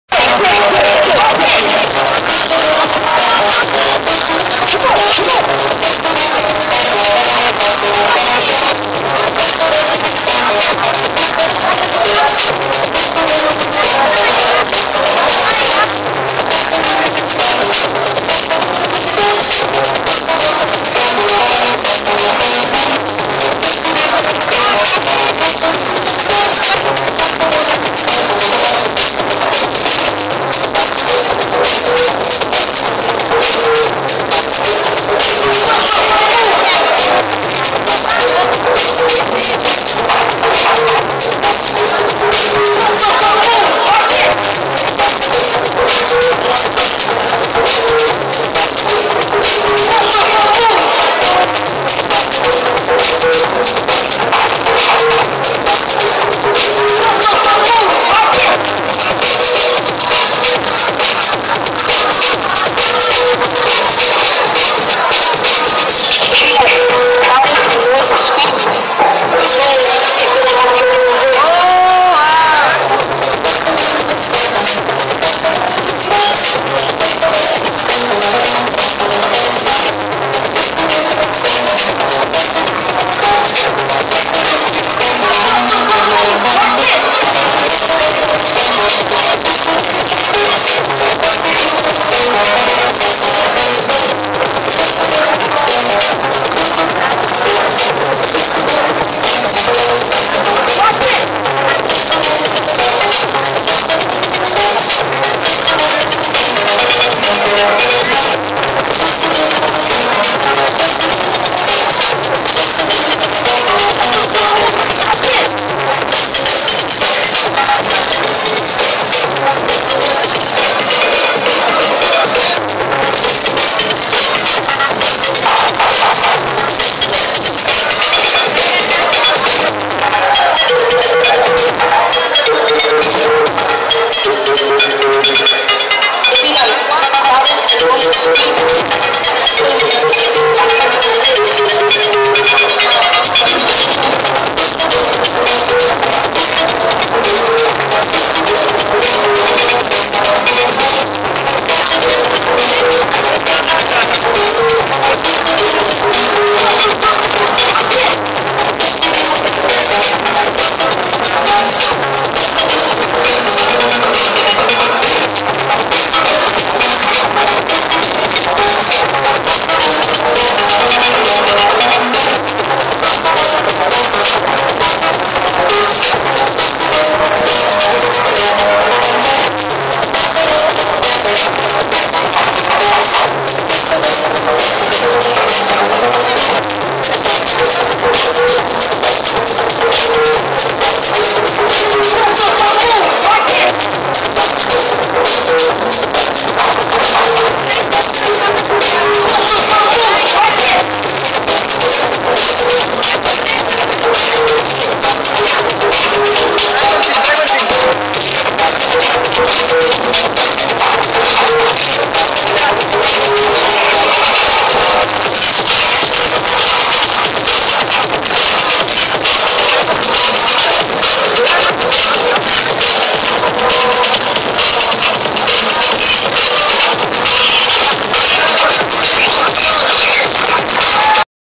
jam-b
fiesta urban retro break   (entrando en accion el gran jam-b) la lio gorda